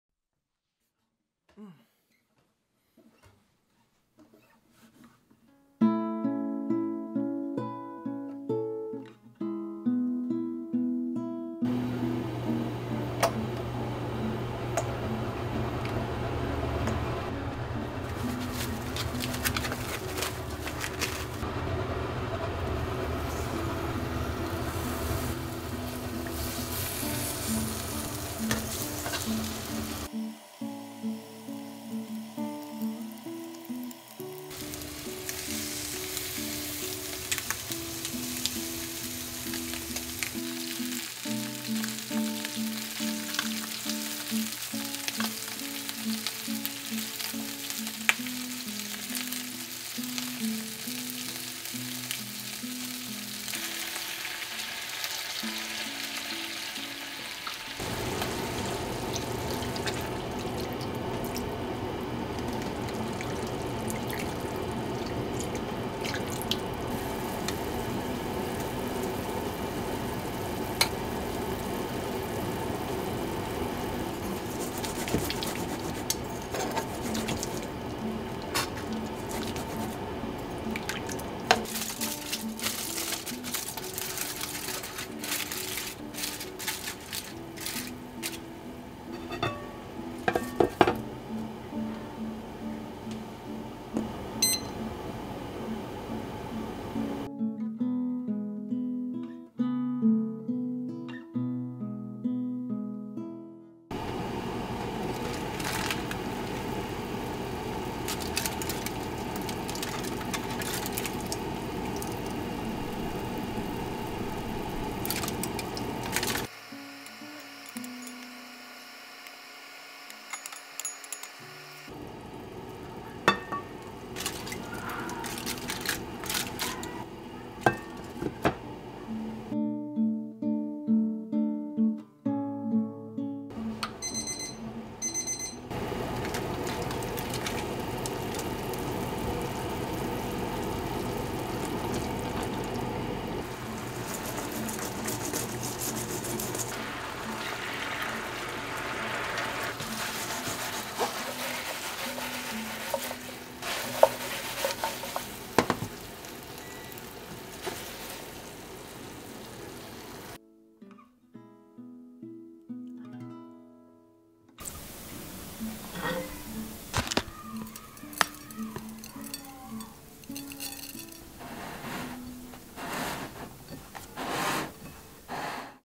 Chicken Wing Stick Recipe | Delicious Japanese Food
Recipe for 3~4 peoples:
16 Chicken wing stick
60 ml Sake
60 ml Soy sauce
50 ml Vinegar
3 tbsp. Sugar
50 ml Mirin
Sesame